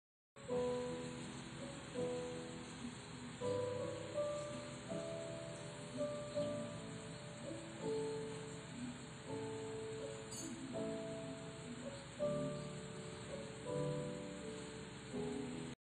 ghost caught on cctv
A ghost was caught on cctv camera look closer from the door i lock by its self the door on the right